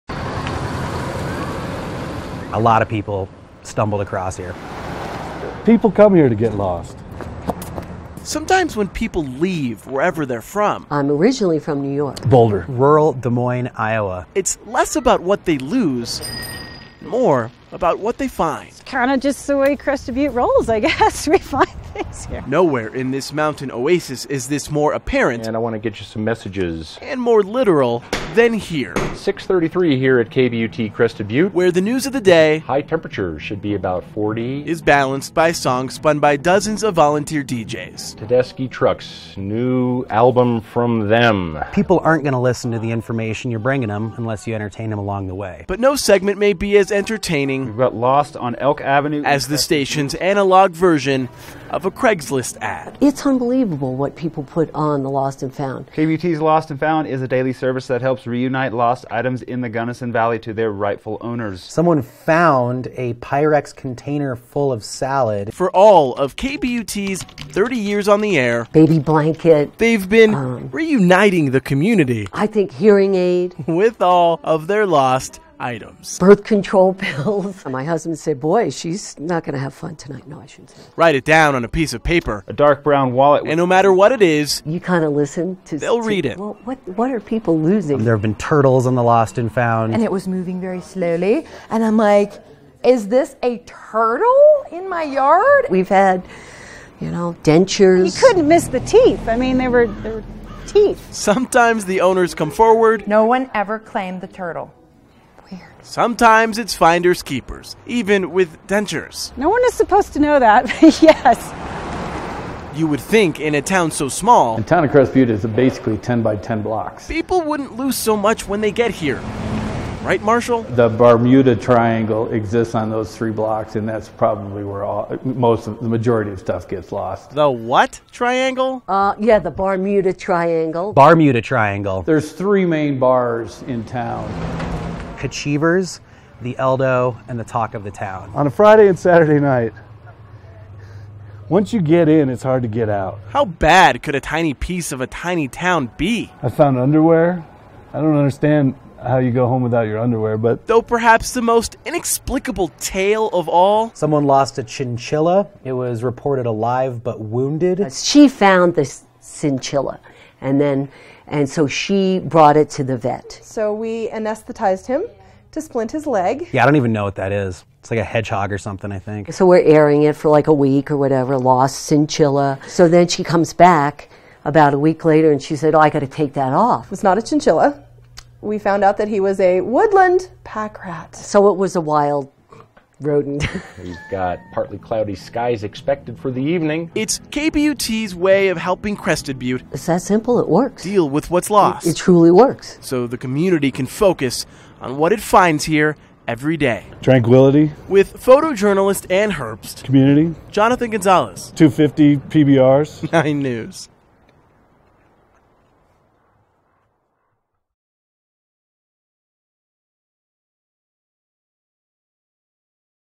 Mountain town radio station broadcasts unique lost and found bulletins
Inside of the KBUT studio, the station’s 1 p.m. volunteer DJ sorts through color-coded pieces of paper, pre-reading them carefully, making sure the writing is legible.